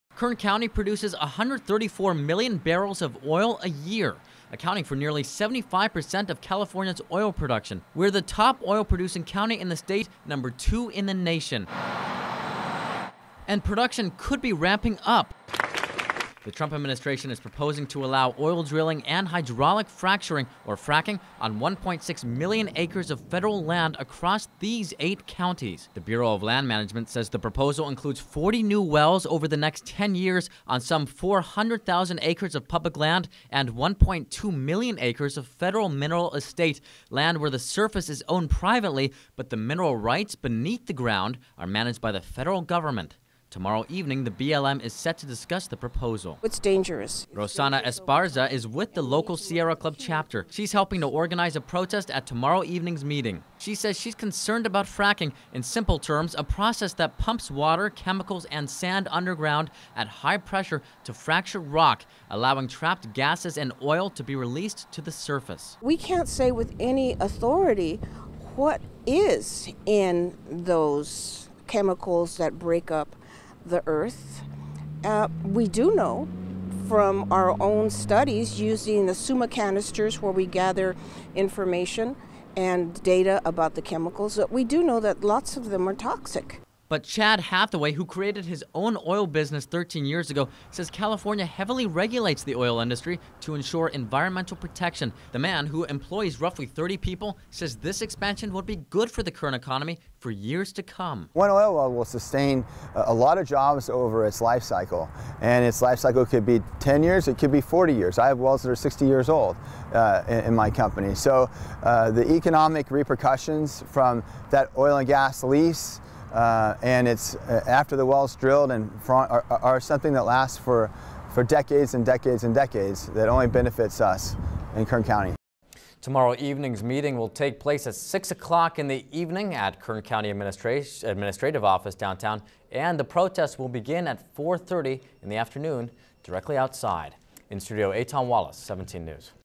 Bureau of Land Management to hold meeting on White House proposal to expand oil drilling, fracking
The proposal would expand drilling on 1.6 million acres of land across the state.